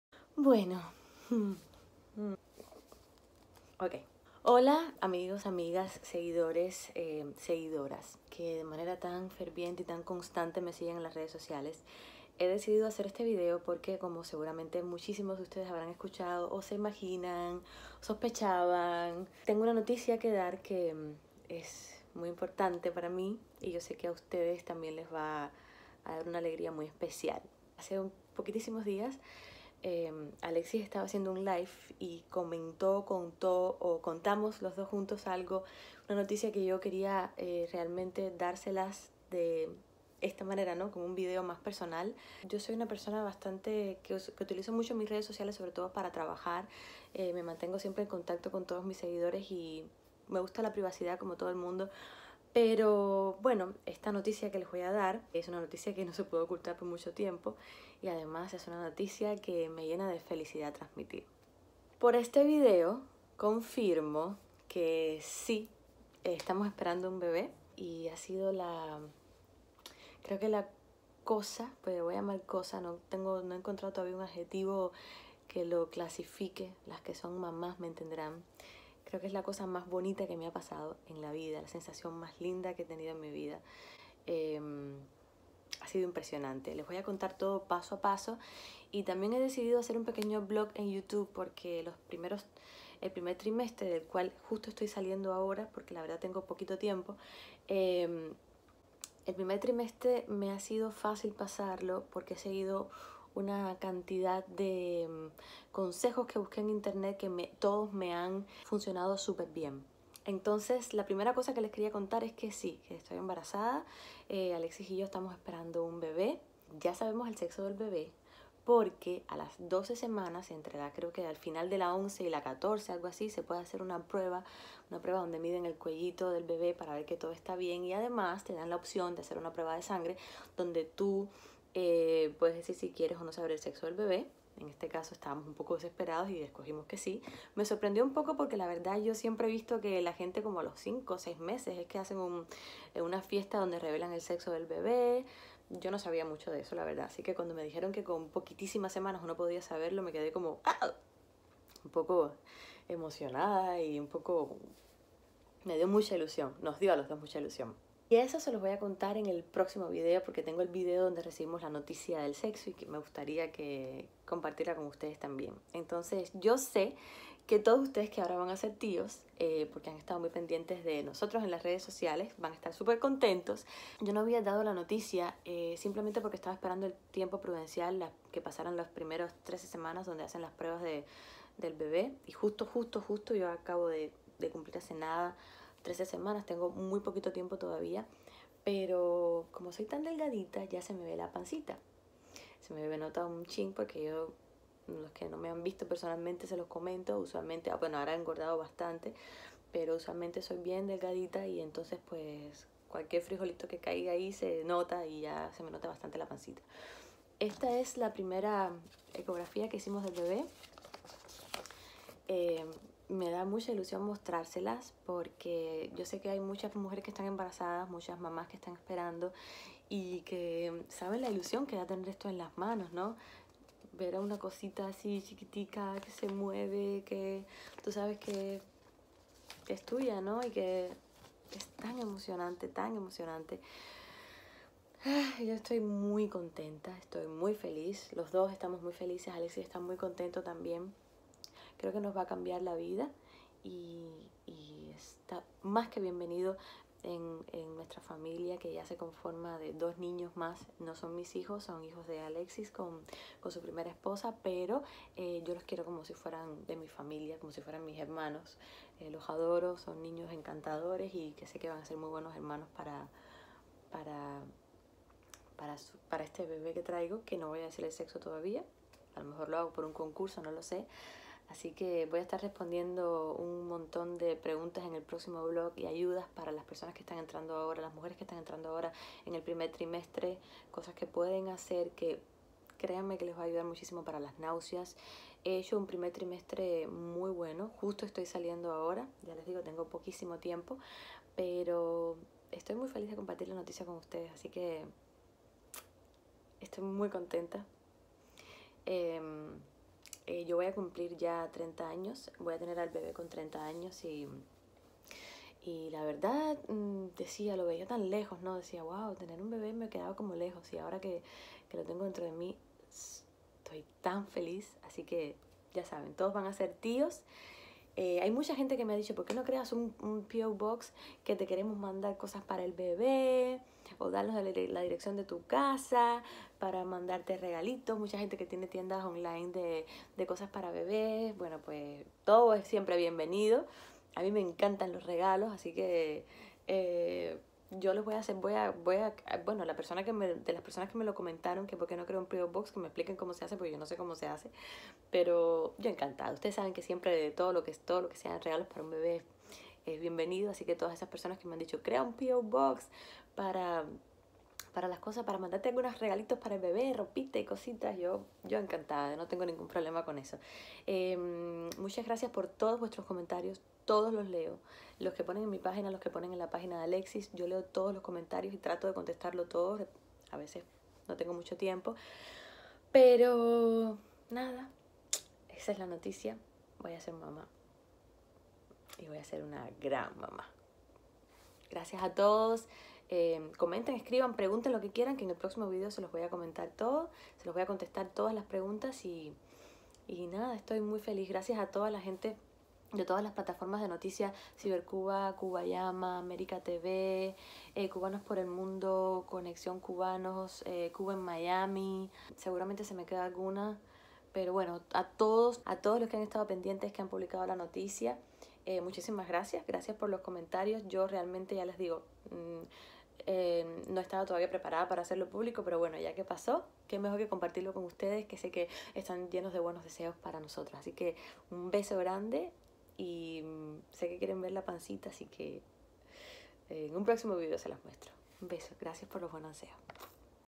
El dia que di la noticia de mi embarazo, solo las madres sabemos lo que se siente!!!
Que emoción!!!